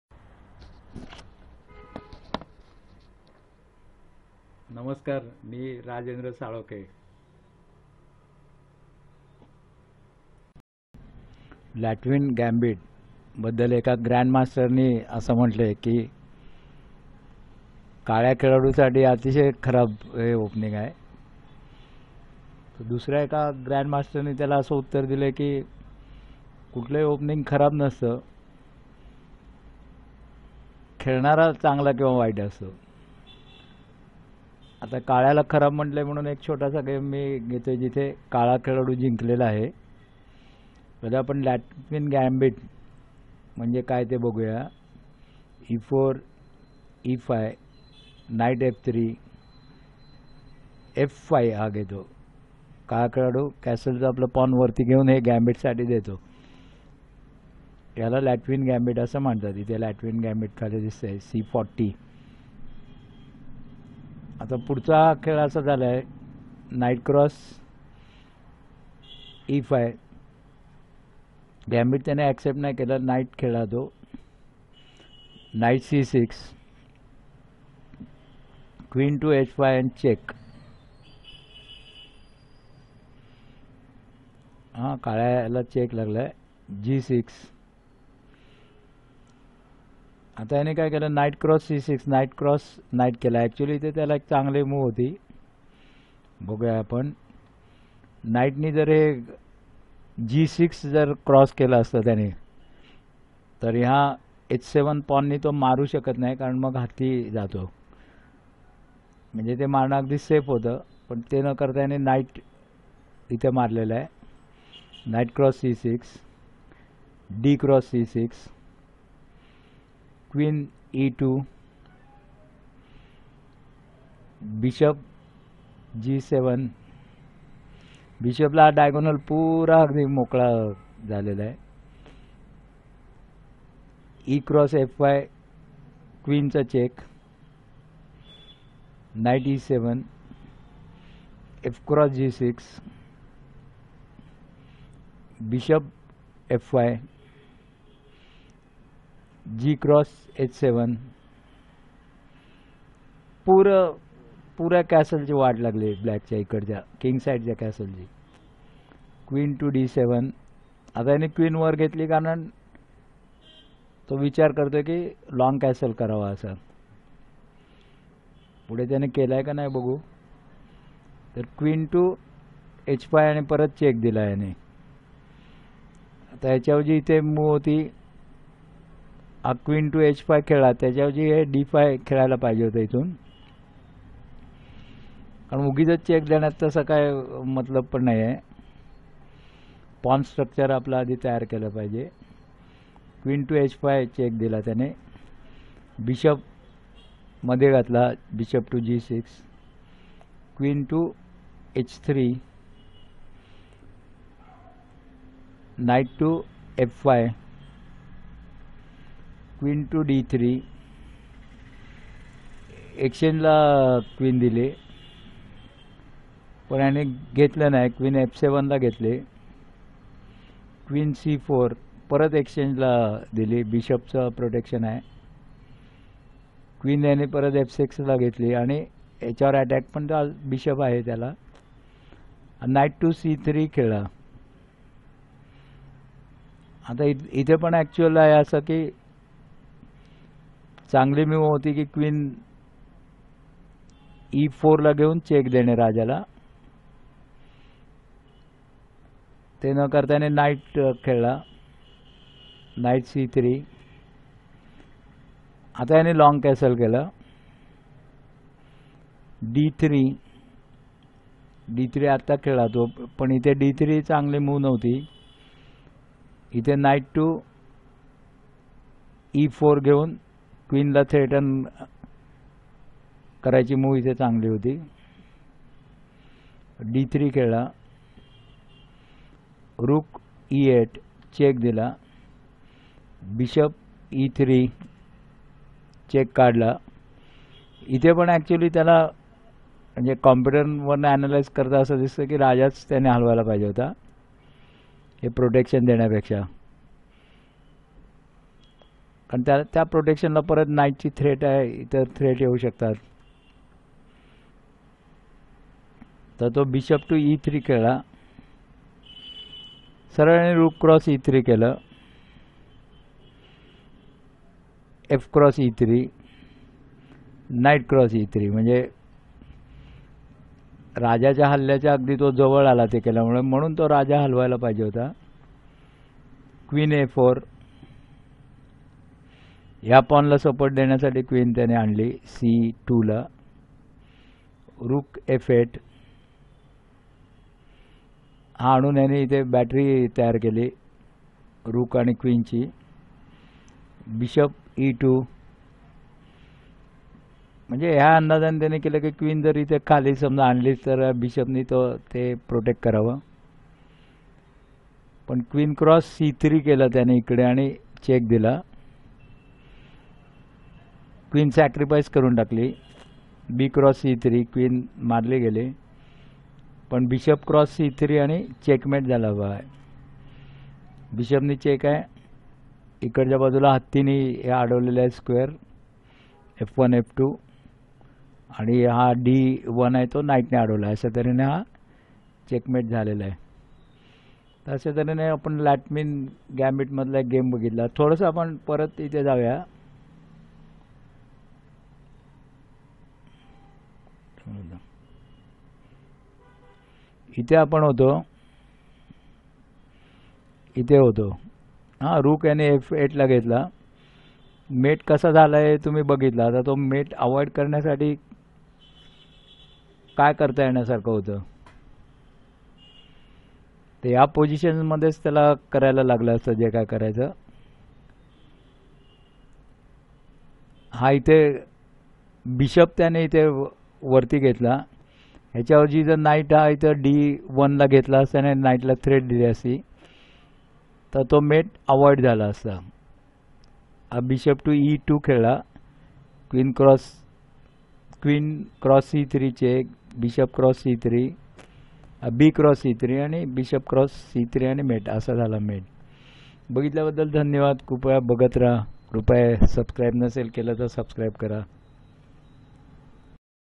Chess Marathi बुद्धिबळ : छोटे खेळ भाग 21.Latvian Gambit.
Latvian Gambit.राजॆंद्र साळोखे
Various chess videos useful for school students.